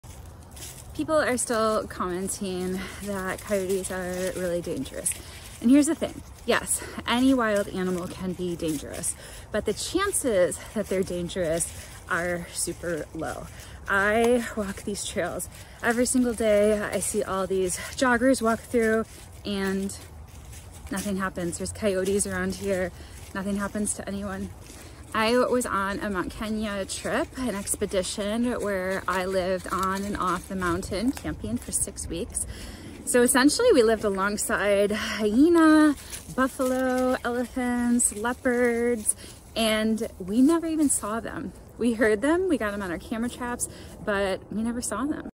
How dangerous are coyotes?
Any wild animal can 100% be dangerous, but the risk is really low. Let me explain and put coyotes in the context of much more dangerous animals that I lived alongside with.